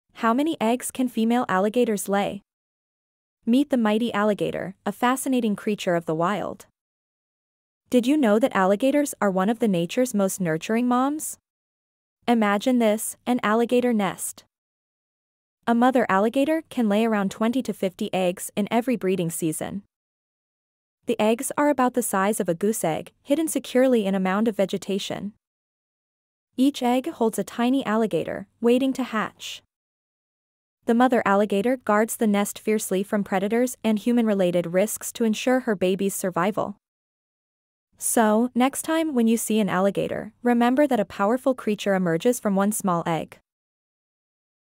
How many eggs can ALLIGATORS lay every year?
ALLIGATOR is one of the most fierce wild animals on earth. You can find a lot of alligators in US and China. However, do you know how many eggs can a female alligator lay during breeding season? Sounds interesting, right? Please take a look at this short video to learn about the interesting fact about female alligators. 🐊🐊🐊🐊